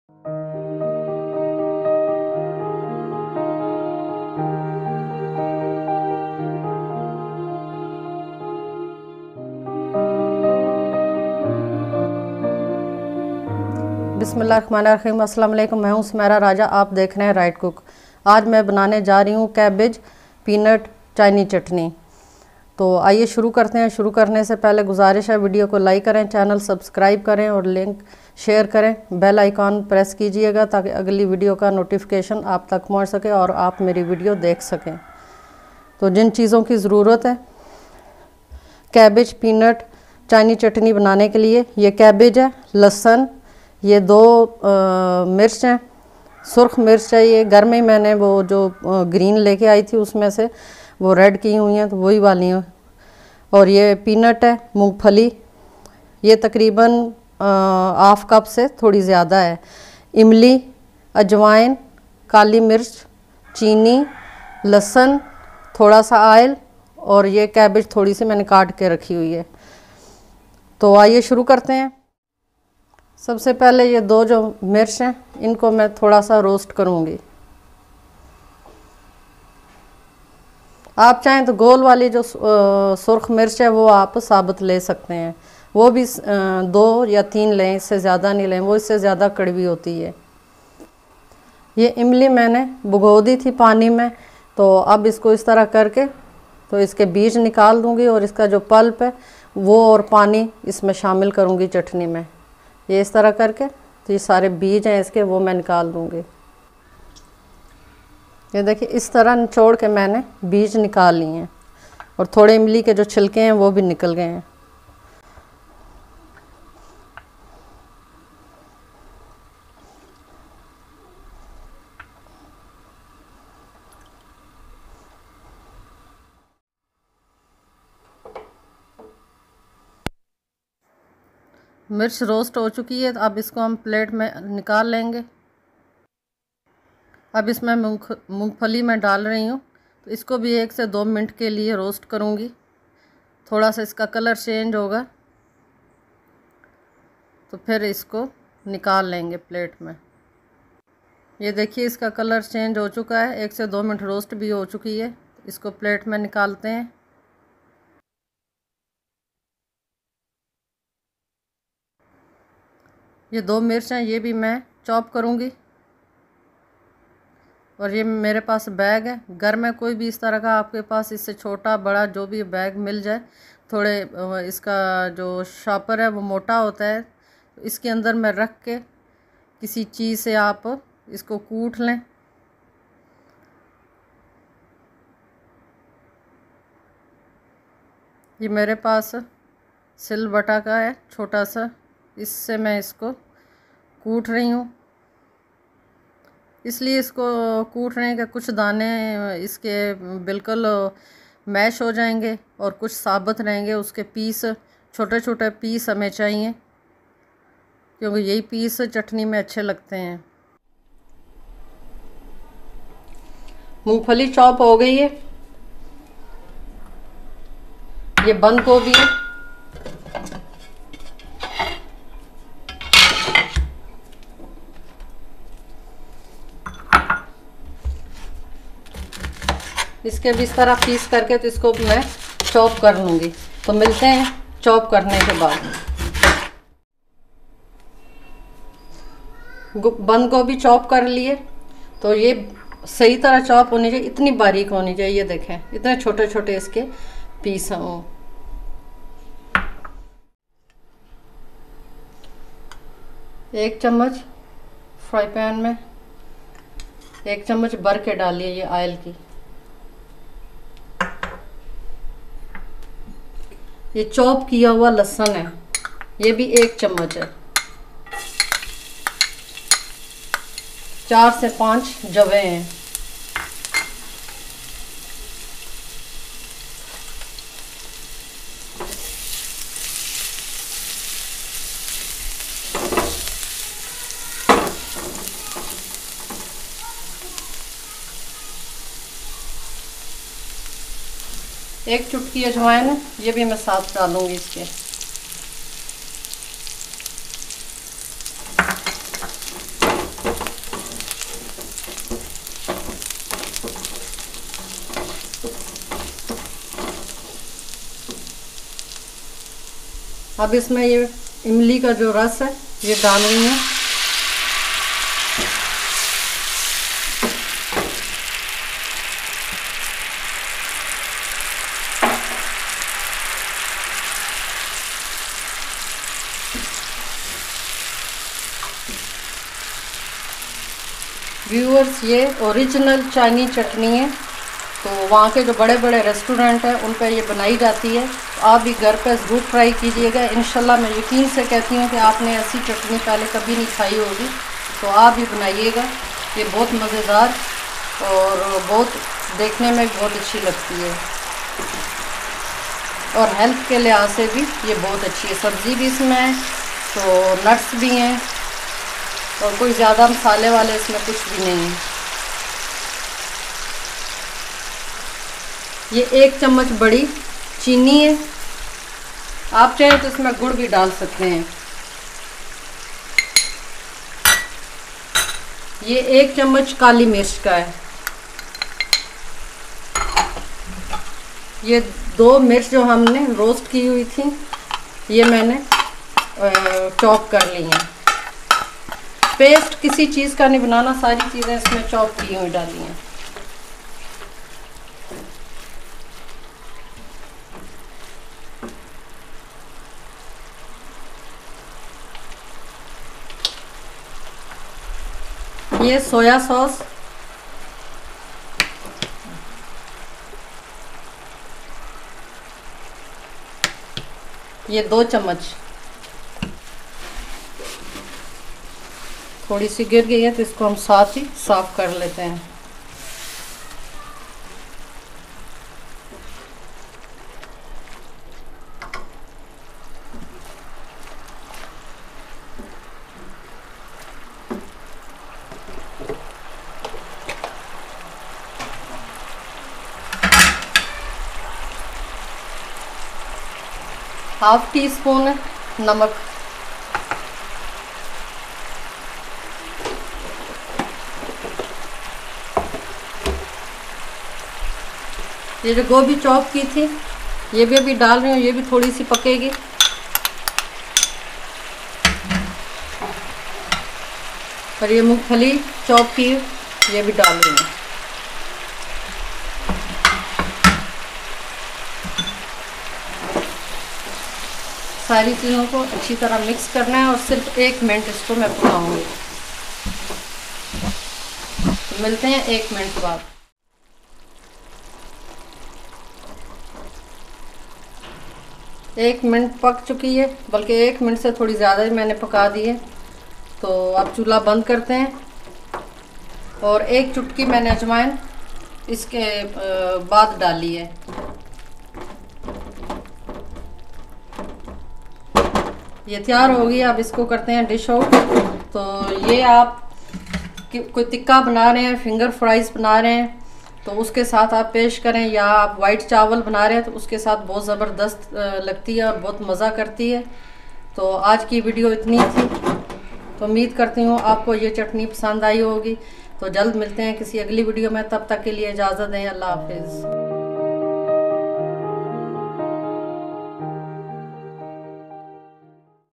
Cabbage and Peanut Sauce at home // Origional chinese hot recipe // Urdu / Hindi
In this video you will watch how to make hot Chinese sauce with cabbage, peanuts, tamarind and spices at home, it has great taste.